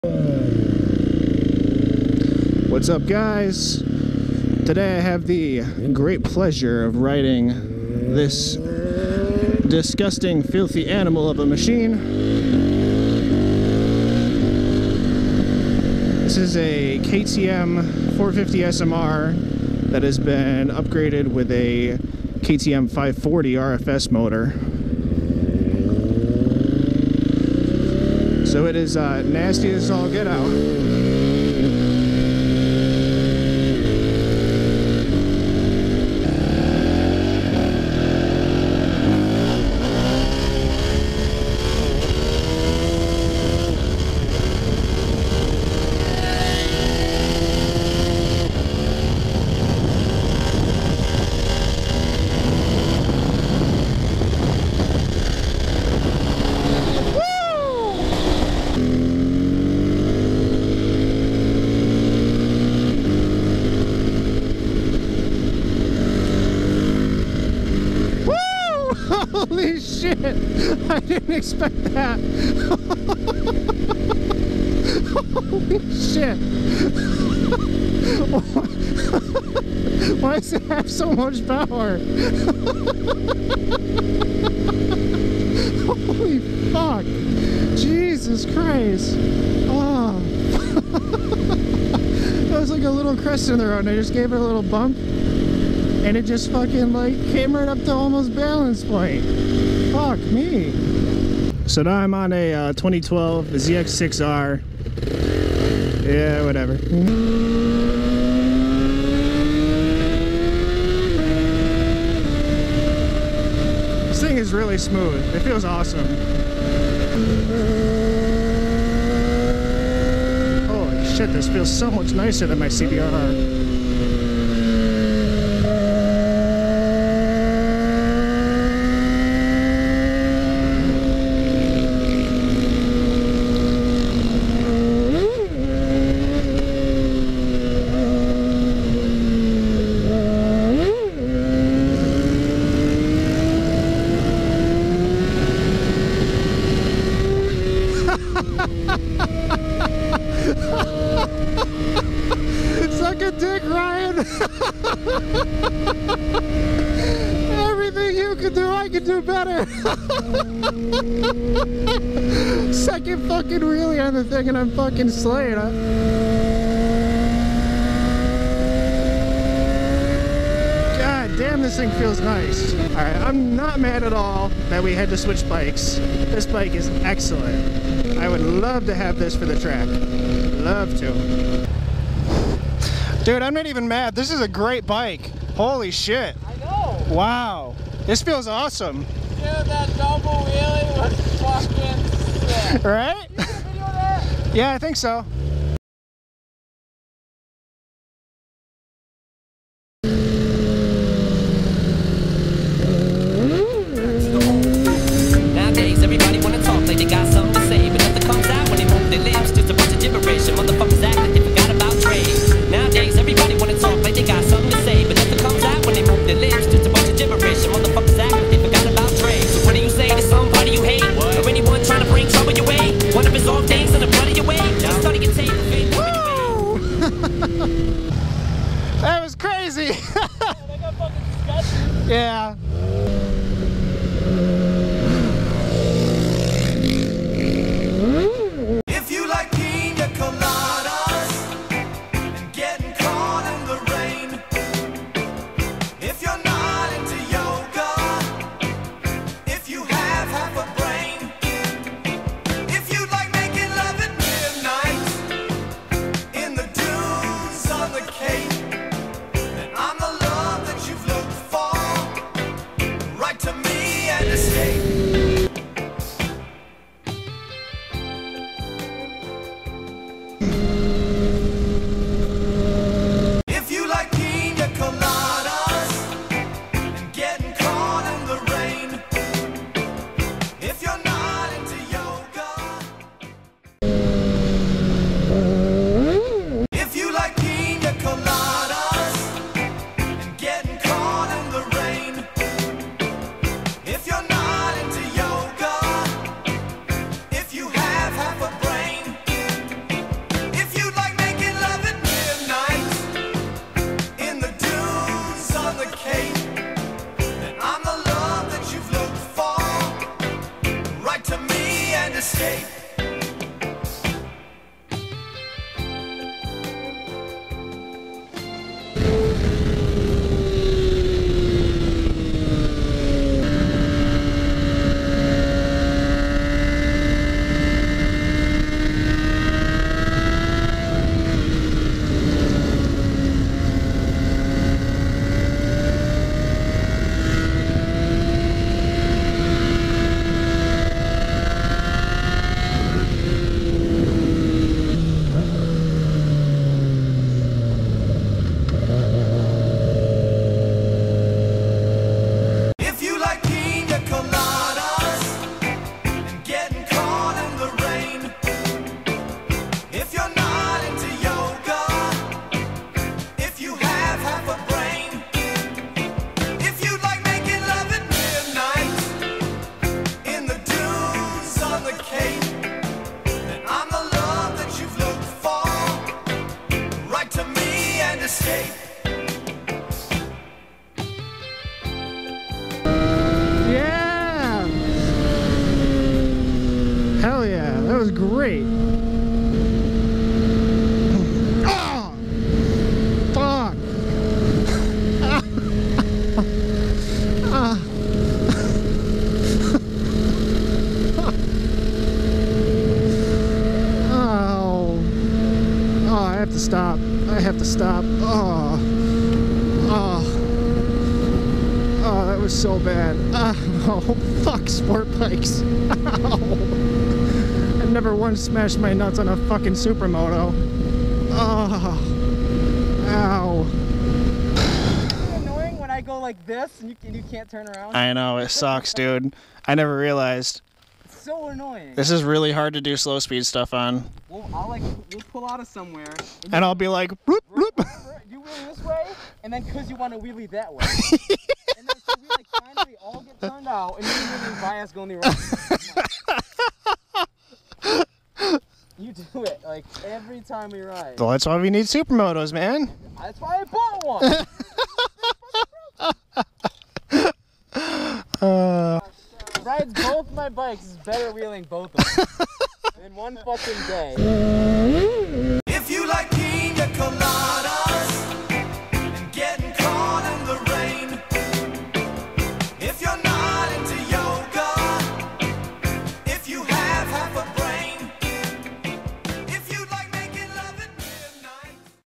Wheelieing everyone elses shit
sometimes you just have to wheelie everyone else's stuff.